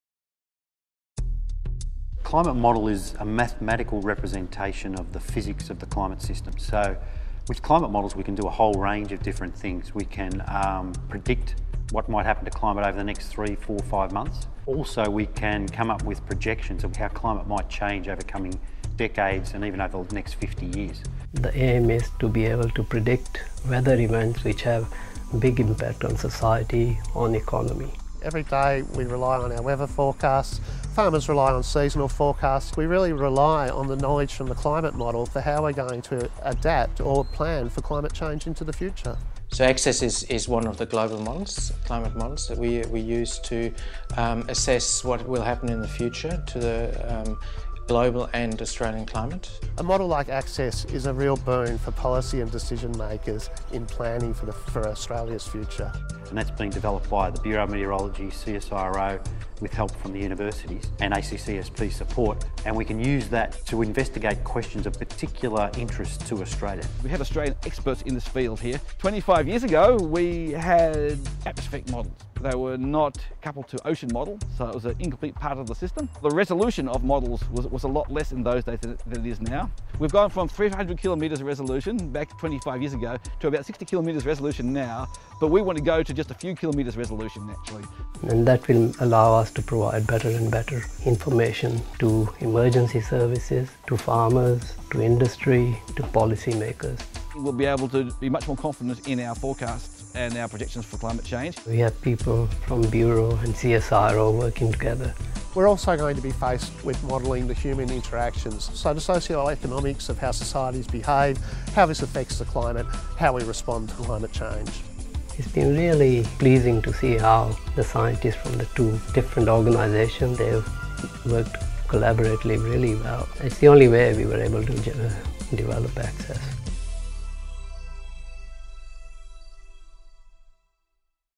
Australian climate model
The Australian Community Climate and Earth System Simulator (ACCESS) helps predict what will happen to our weather in the coming months and provides projections of our climate decades ahead.

The model is a collaboration between CSIRO and the Bureau of Meteorology.